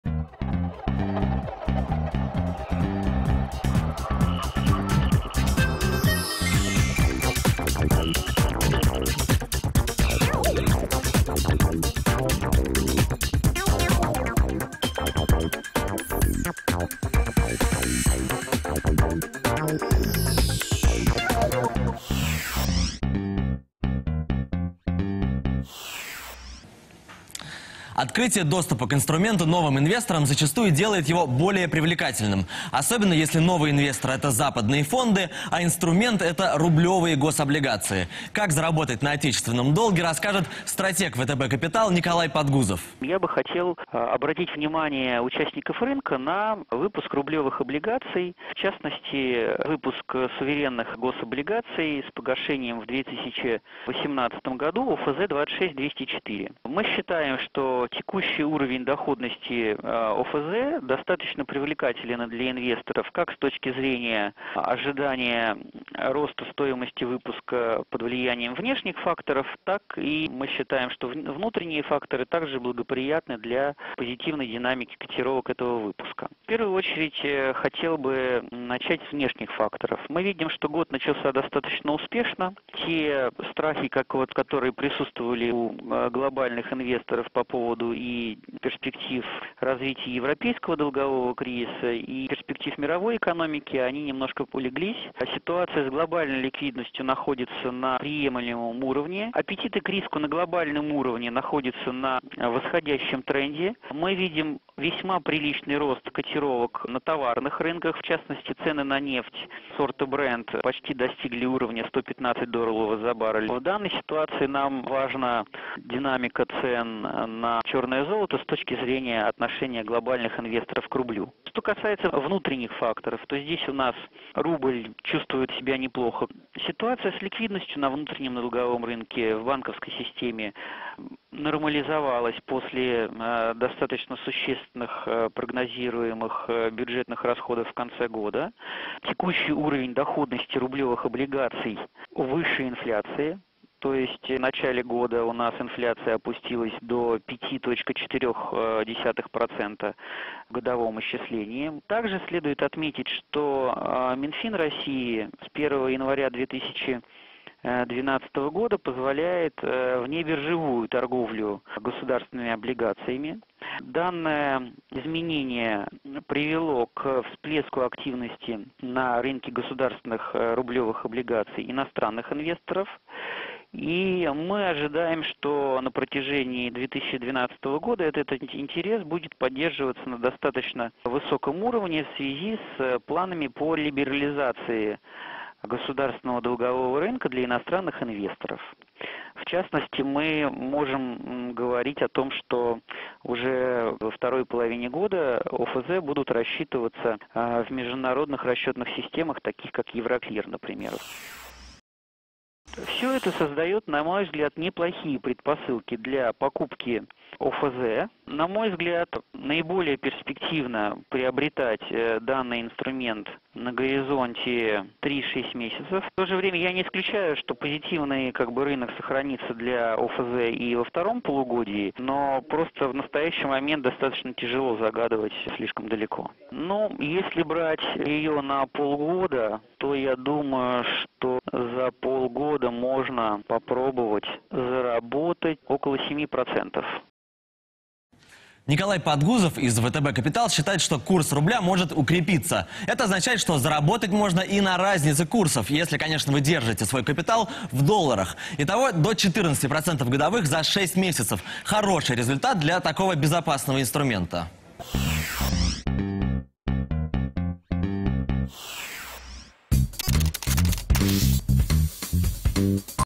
ОФЗ -- доходность до 14% годовых за полгода
ИДЕЯ ДНЯ. Как заработать на отечественном долге, рассказал Николай Подгузов, аналитик «ВТБ Капитала».